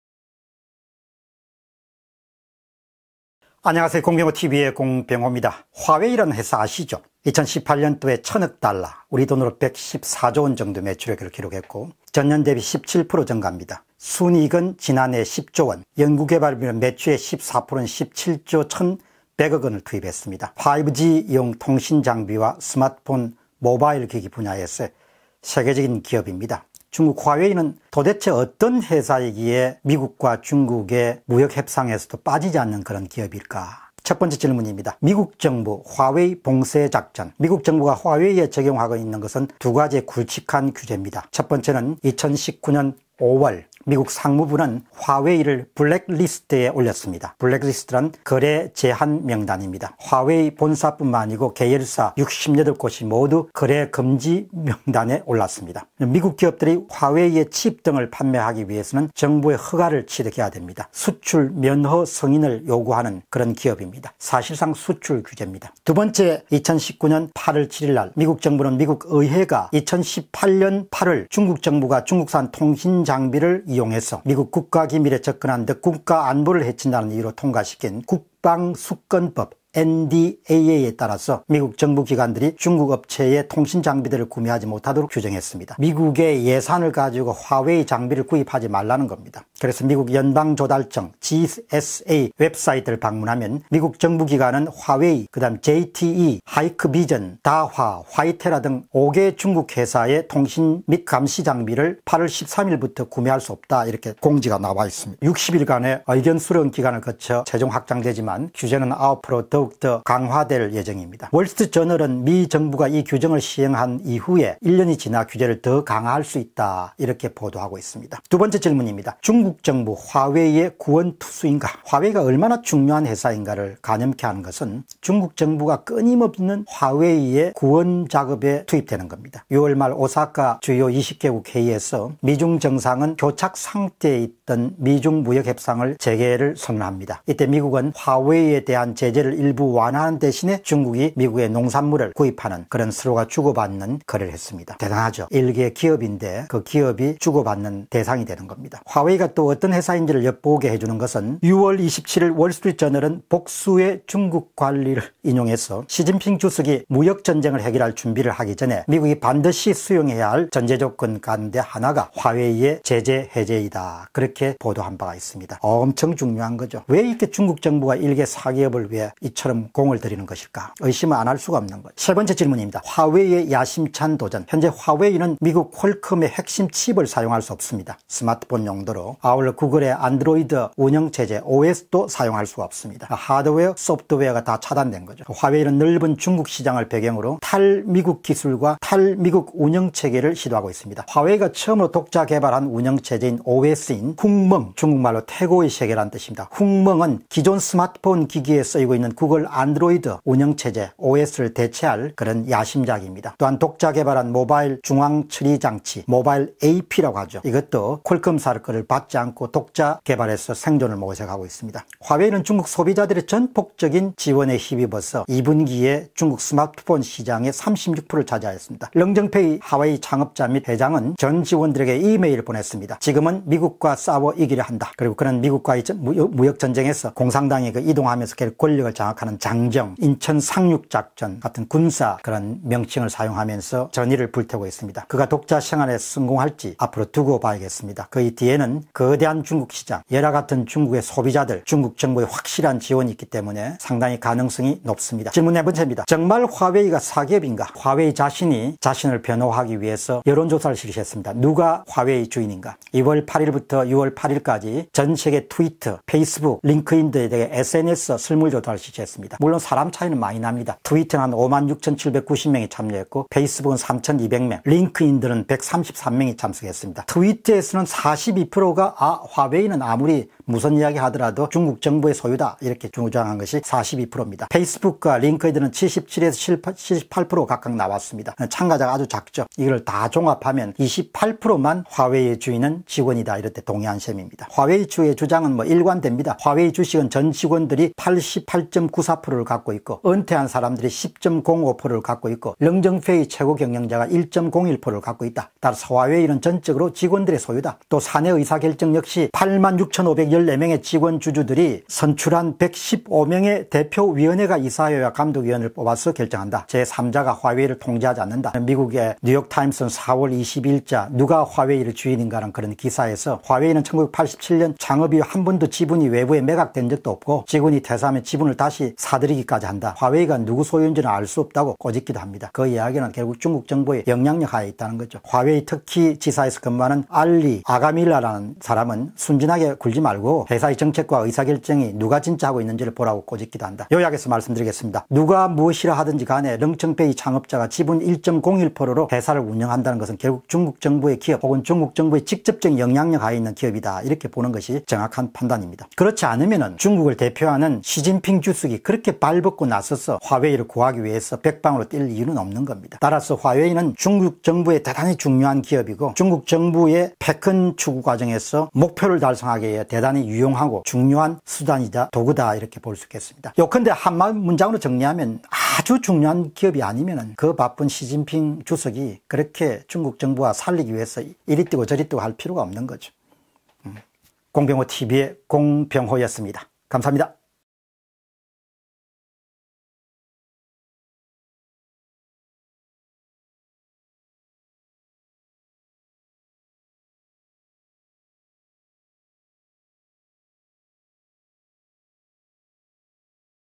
화웨이, 미국과 맞짱뜨기 [공병호TV]
화웨이,
미국과 맞짱뜨기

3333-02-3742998 카카오은행 (예금주 : 공병호) / (텔레뱅킹 : 090 & 은행코드 : 90)

참고자료
1. 화웨이 주인은 누구? 무성한 뒷말에 중국 화웨이 셀프 팩트체크 보니, 동아일보, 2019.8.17.
2. 화웨이, 미국 제재에도 기술 굴기, 탈미국 생태계 확보 가속, 윤민혁, 조선일보, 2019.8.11.
3. 화웨이 회장, 미국과 싸워 이기려면 인천상륙잔적 기억해라, 이민정, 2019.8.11. 중앙일보
4. 화웨이 미국 견제에도 지난해 매출 1000억달러 훌쩍, 이광영, 조선일보, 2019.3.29.
5. 미 상무부, 화웨이 제재 여전 블랙리스트 포함, 김수영, 한국경제, 2019.7.4.
6. U.S. Plans Temporary Exemptions to Huawei Blacklist, John D. McKinnon, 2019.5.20